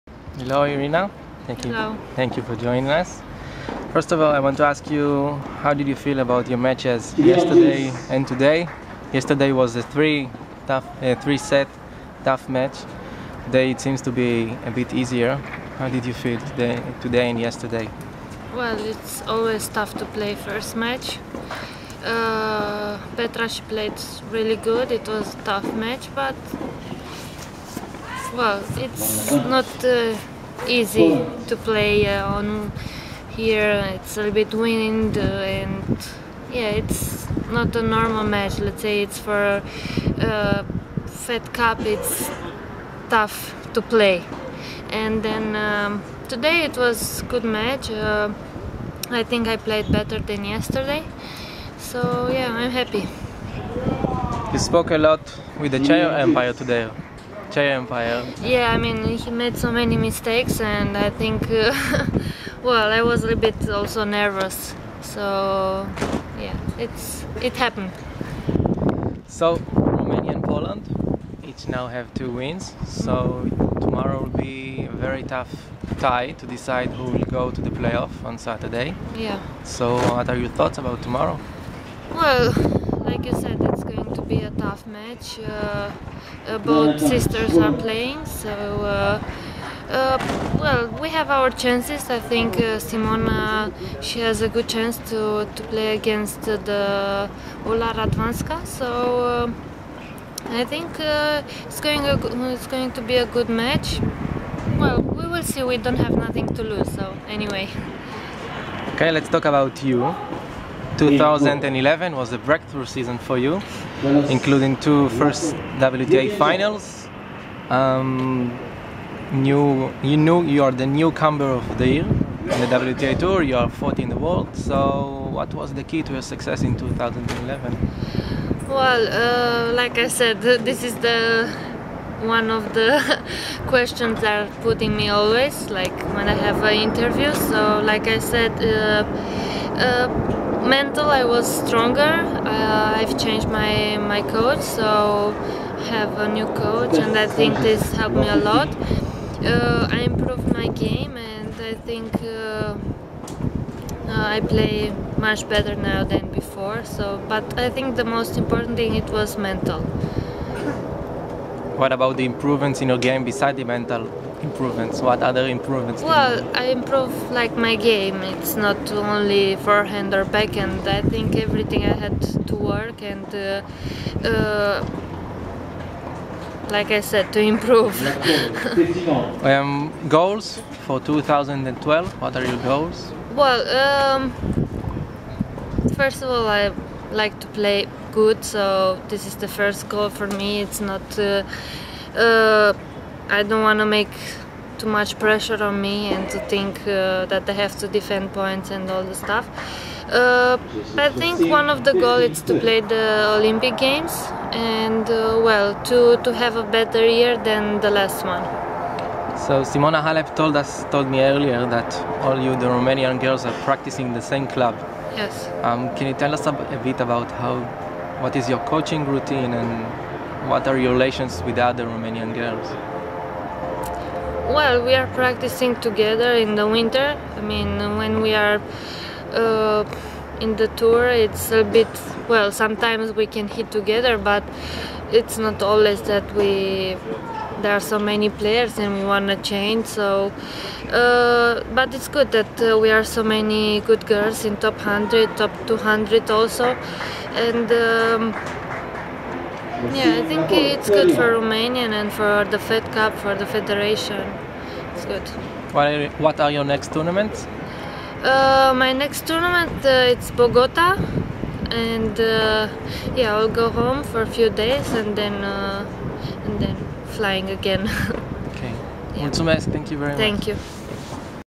Irina-Camelia Begu Fed Cup 2012 interview
Irina-Camelia Begu is giving an interview for Israel Tennis Results, during the 2nd day of the Group I 2012 Europe/ Africa. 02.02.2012.